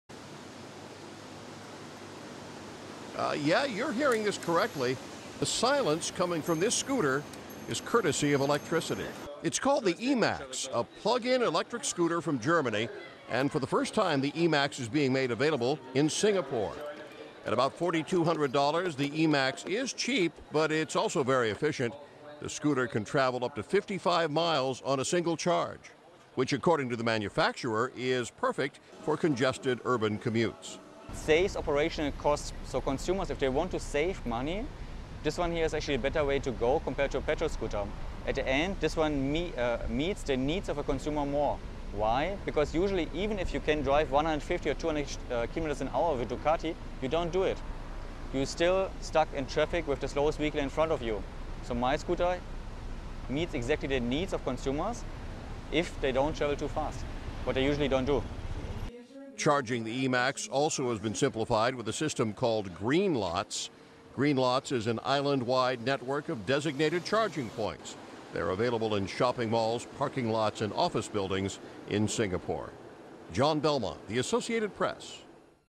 Electric Scooter Targets Urban Commuters
A popular electric scooter has made its way to Singapore, where it could prove to be a zippy way to commute.(Aug. 26)